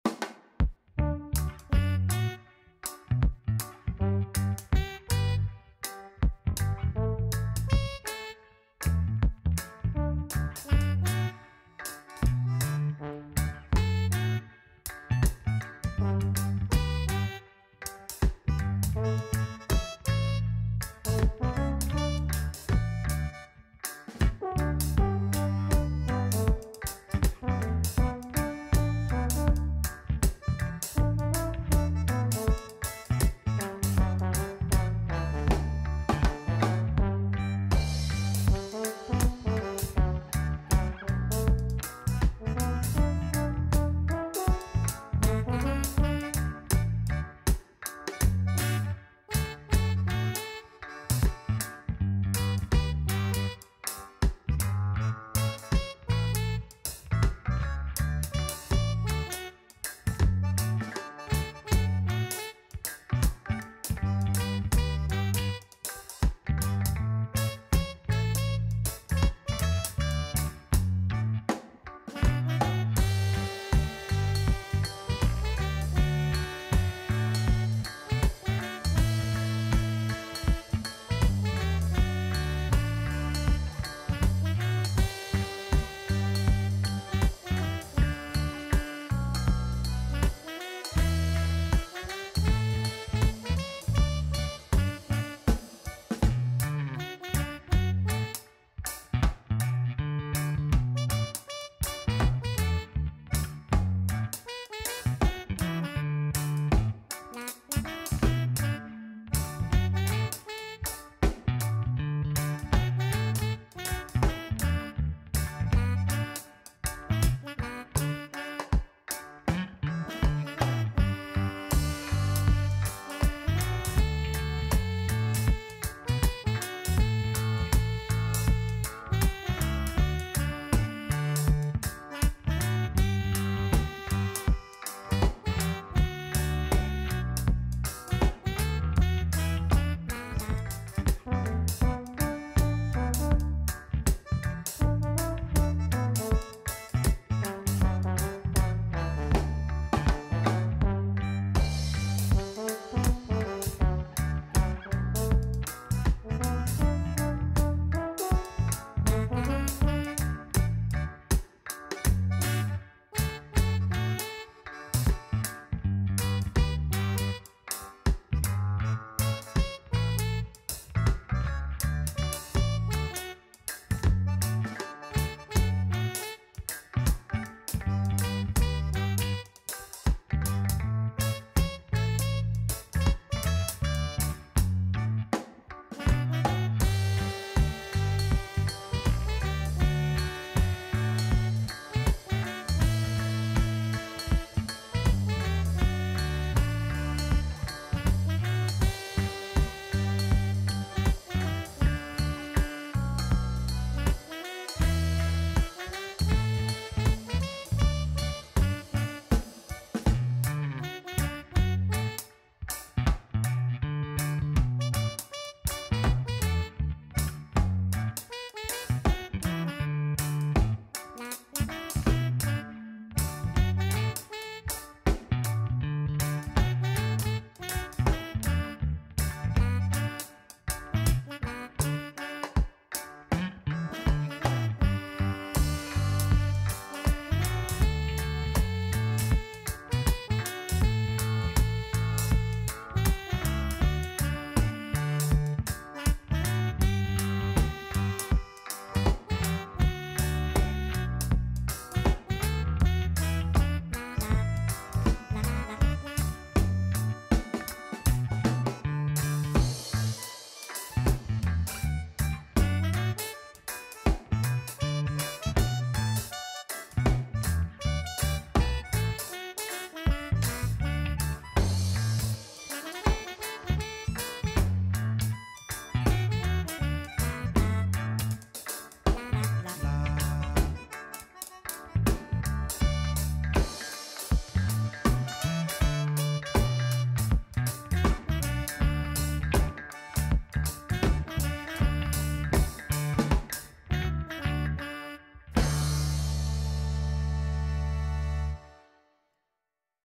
Drawing Banana Bomb - Procreate - iPad Pro - Speedpaint
Drawing a banana doing standup comedy.

Drawn using an iPad Pro (2018) Apple Pencil & Procreate 5.

By:   Quincas Moreira